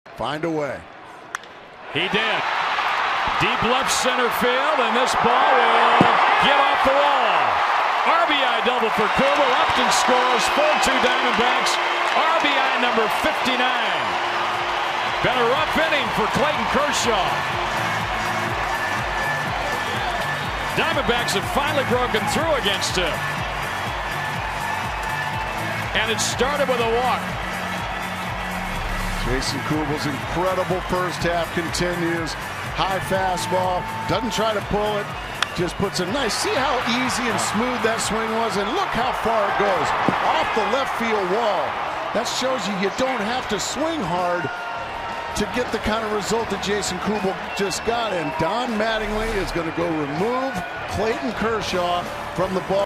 2012/07/06 Kubel's RBI double
LAD@ARI: Kubel doubles D-backs lead with RBI double